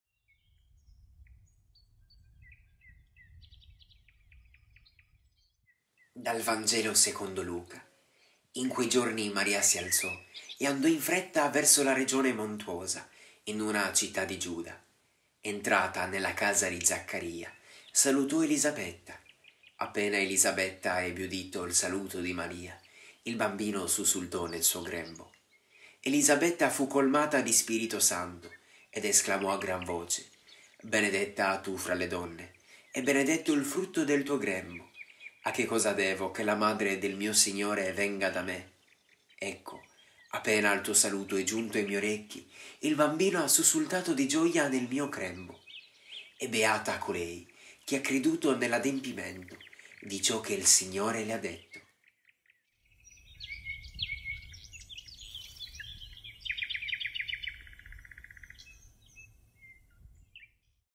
Vangelo del giorno - Feria d'Avvento - Lunedì 21 Dicembre 2020 - Lc 1,39-45
In quei giorni Maria si alzò e andò in fretta verso la regione montuosa, in una città di Giuda.
Entrata nella casa di Zaccarìa, salutò Elisabetta. Appena Elisabetta ebbe udito il saluto di Maria, il bambino sussultò nel suo grembo.
Elisabetta fu colmata di Spirito Santo ed esclamò a gran voce: «Benedetta tu fra le donne e benedetto il frutto del tuo grembo! A che cosa devo che la madre del mio Signore venga da me? Ecco, appena il tuo saluto è giunto ai miei orecchi, il bambino ha sussultato di gioia nel mio grembo. E beata colei che ha creduto nell'adempimento di ciò che il Signore le ha detto».